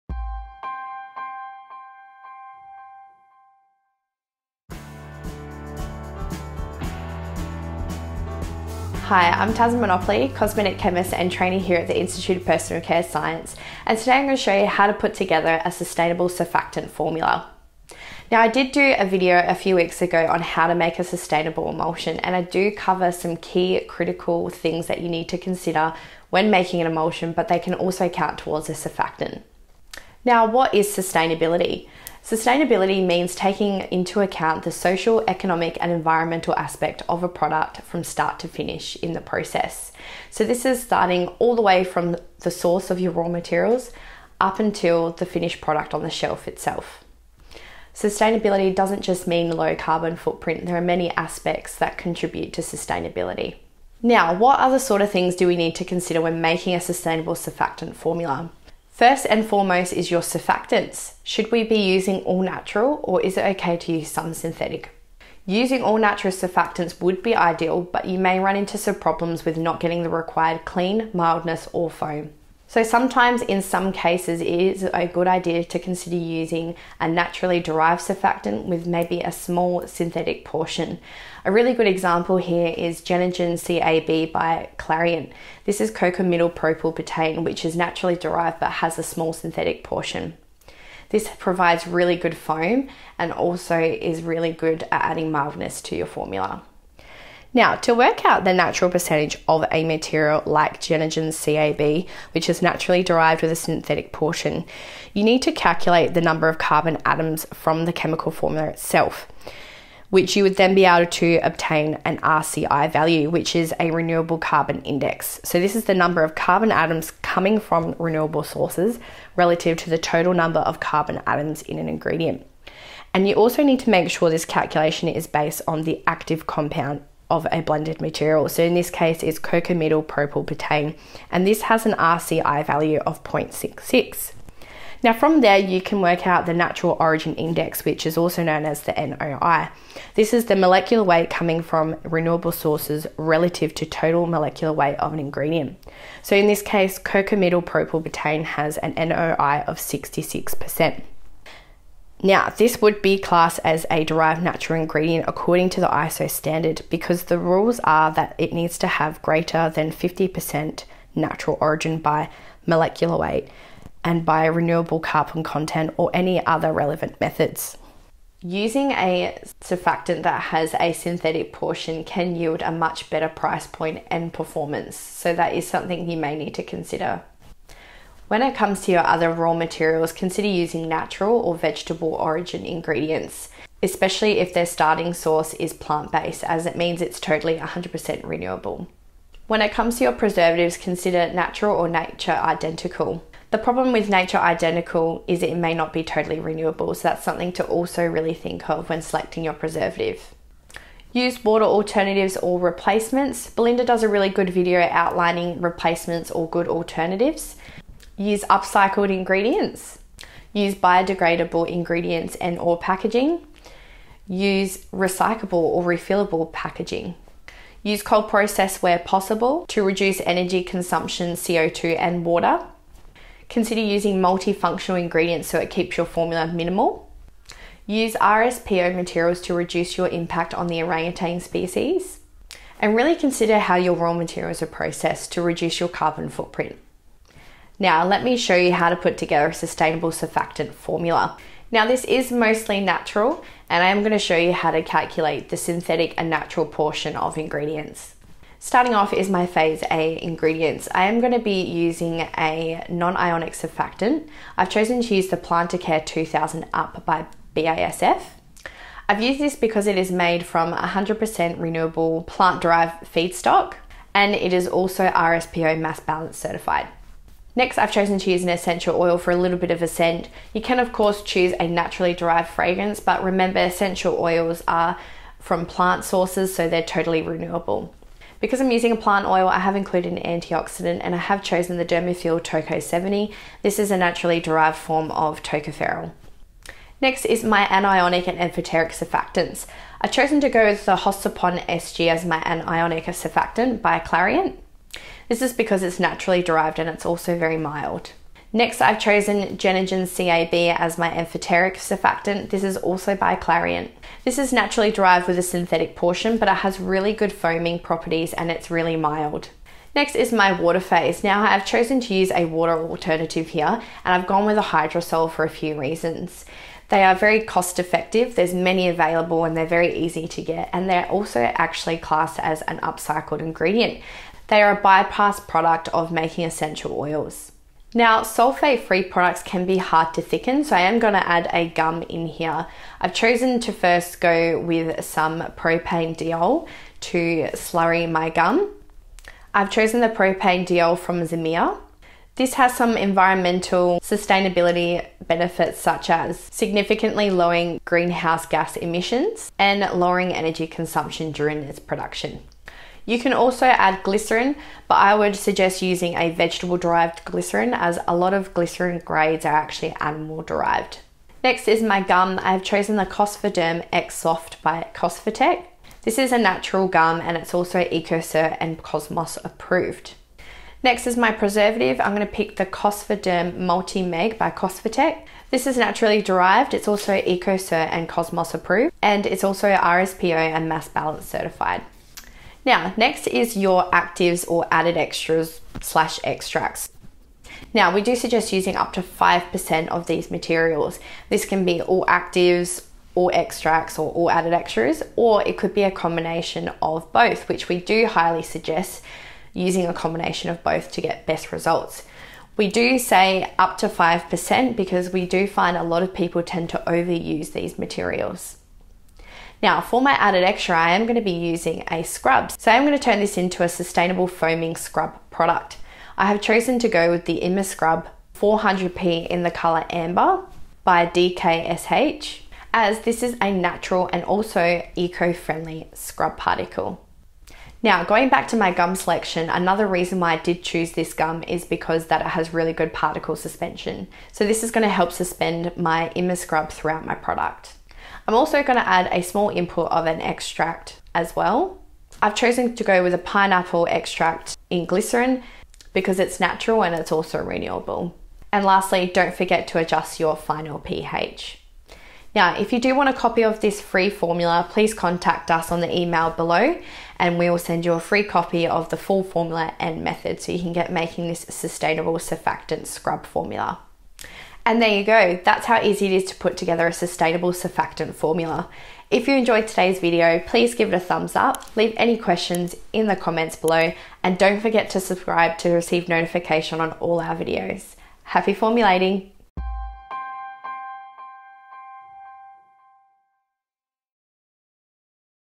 How to create a sustainable surfactant formula - foaming scrub
Want to make your own sustainable surfactant formulas but not sure how? In this video, we show you how to write up your own sustainable foaming formulas, with a formula you can start with, and talk you through the essential elements of making it sustainable formula.